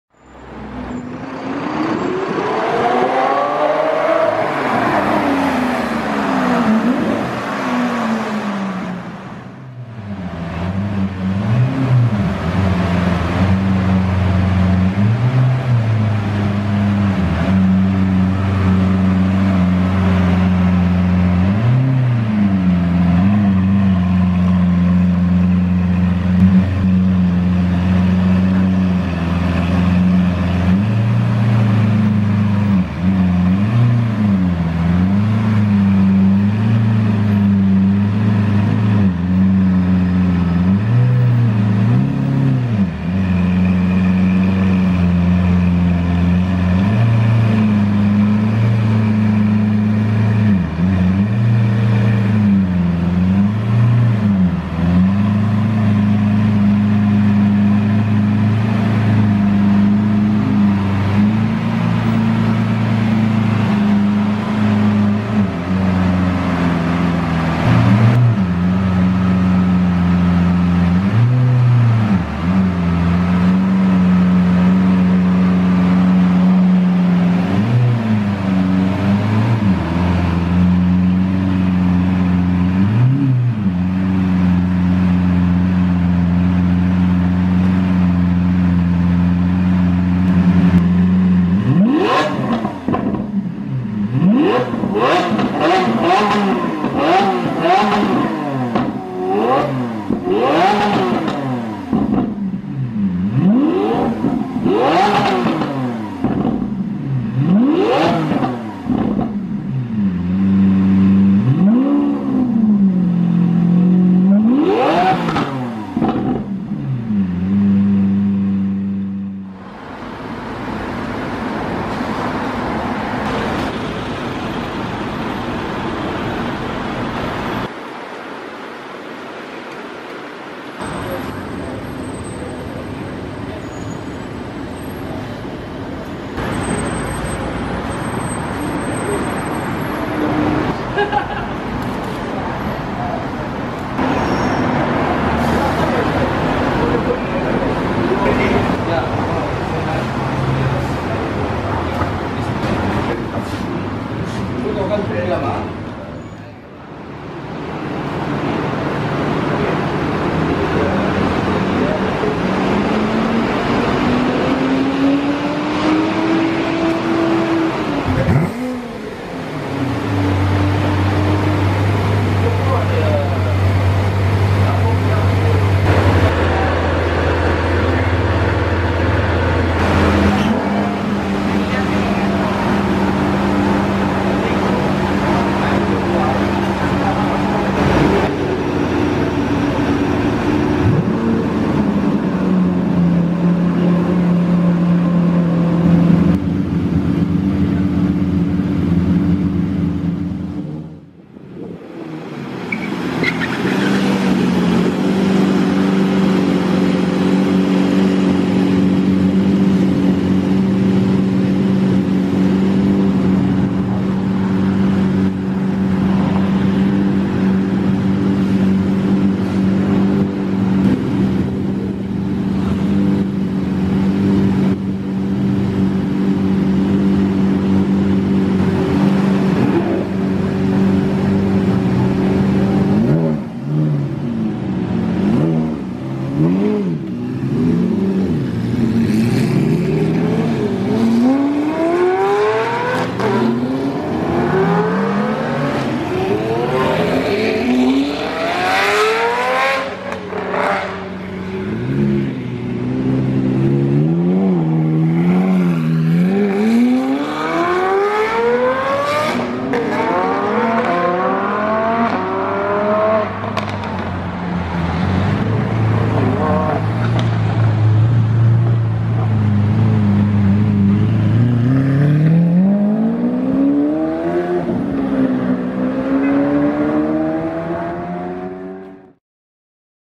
Lamborghini Owners Club Malaysia Accelerations + DMC LP900-4 REVS!
A group of bulls from the Lamborghini Owners Club Malaysia were here at The St. Regis hotel in Singapore to watch the Singapore F1 Grand Prix.
The star of the show was definitely ML's chrome red DMC Aventador LP900-4 SV which performed some quick revs for the spotters.